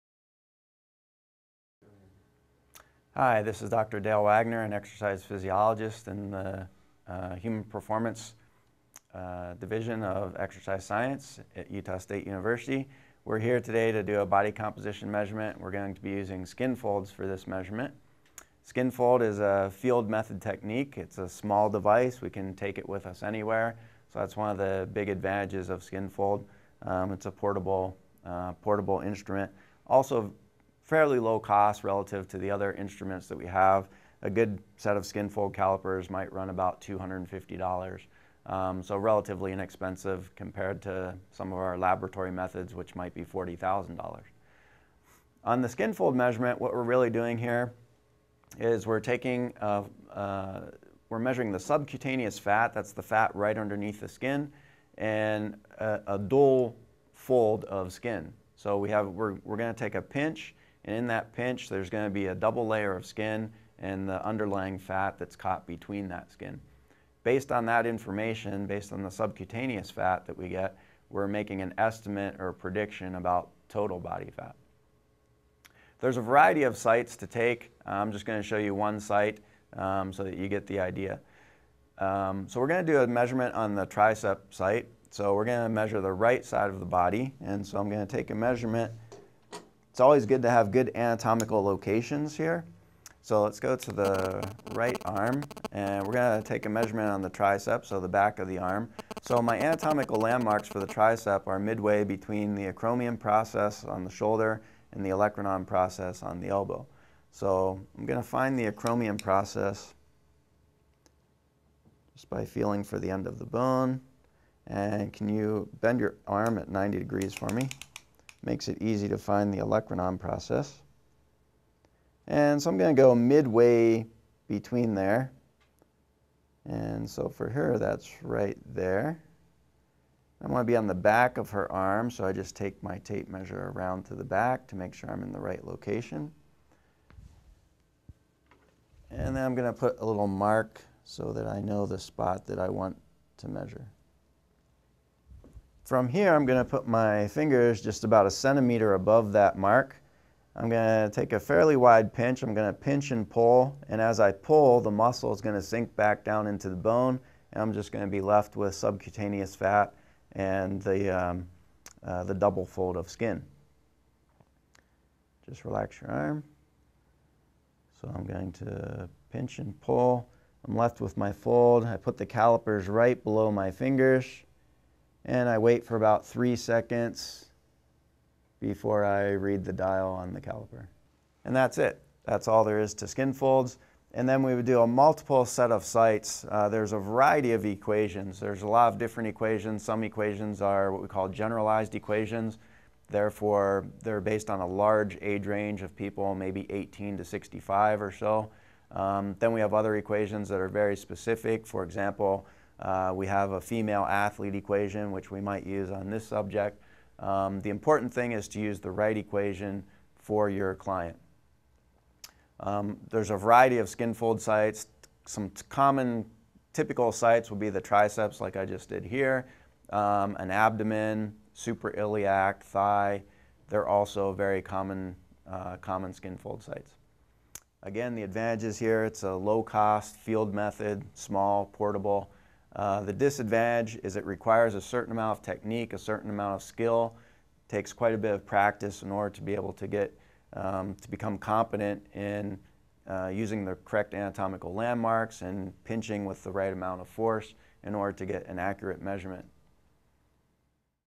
Skin Fold Measurement with Calipers
Learn about the skin fold measurement and what can be observed from this practice.

Connect with USU Extension!